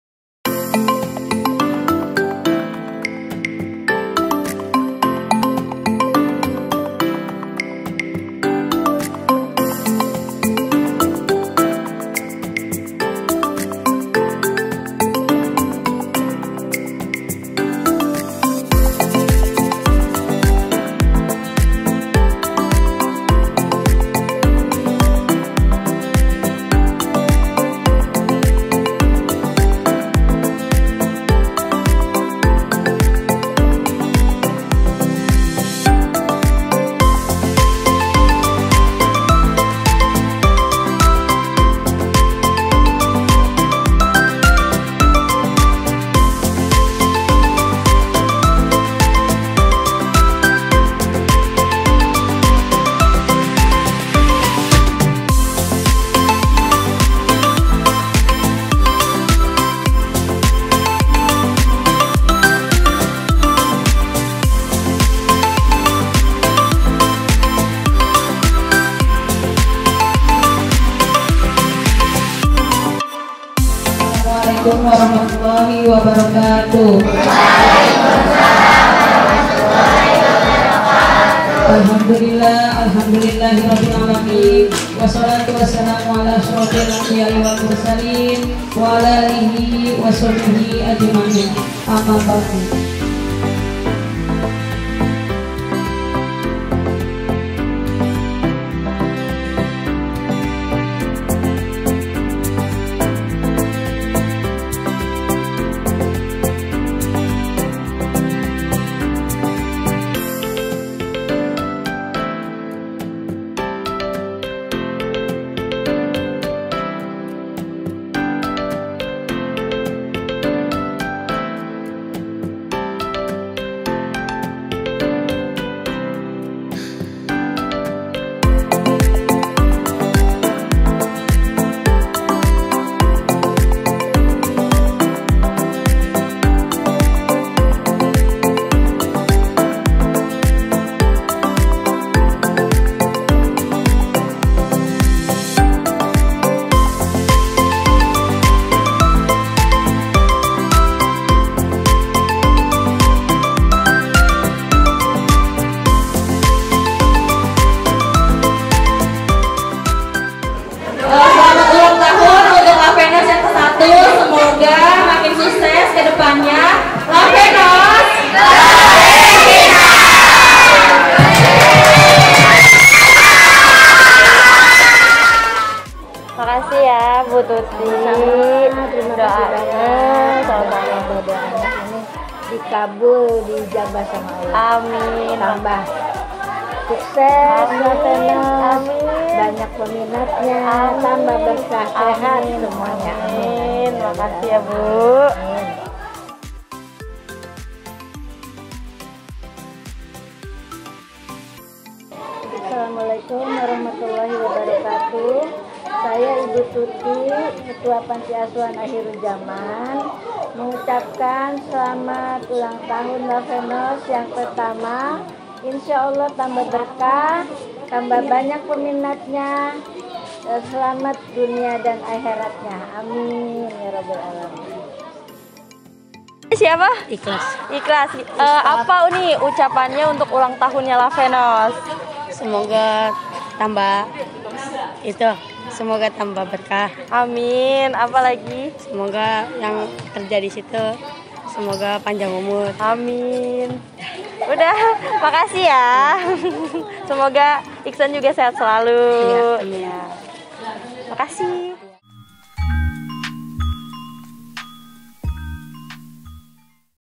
RAYAKAN ANNIVERSARY, LAVENNOZ BERBAGI BERSAMA ANAK YATIM
Lavennoz Berbagi Bersama Anak Yatim

Dihari ulang tahun Lavennoz yang pertama, Dirayakan dengan berbagi bersama anak Yatim Piatu di Panti Asuhan Akhiruz Zaman kota Bekasi.

Semoga lavennoz tambvah sukses lagi, Barokah dan terus menebar manfaat kebaikan bagi orang banyak...

Lavennoz Beauty Is You, Royalty For YOu